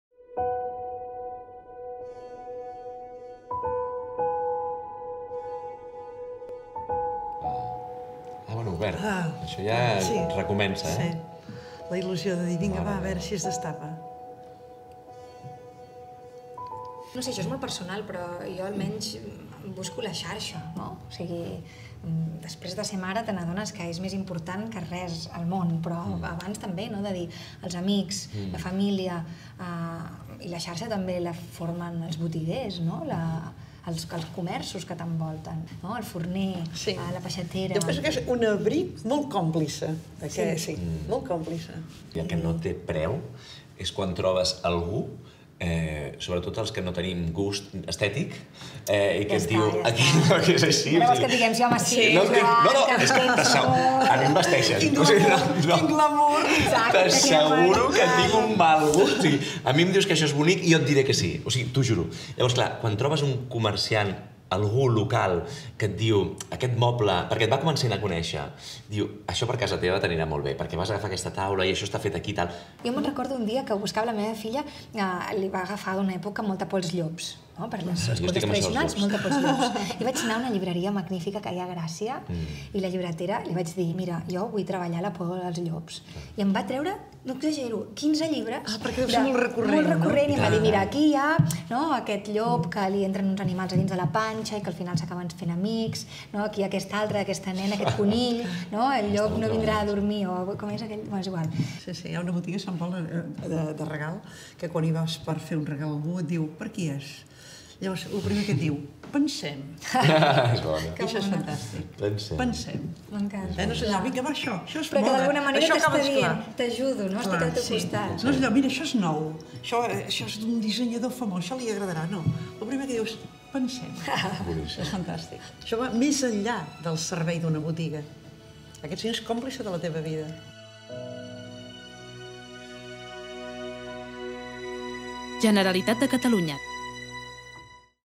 'Dona corda al comerç' amb Joan Dausà, Aina Clotet i Carme Ruscalleda
Joan Dausà, Carme Ruscalleda i Aina Clotet conversen sobre les bondats del comerç de proximitat en aquest vídeo, titulat 'Dona corda al comerç' i emmarcat en la campanya 'Tot queda a casa', del Consorci de Comerç, Artesania i Moda.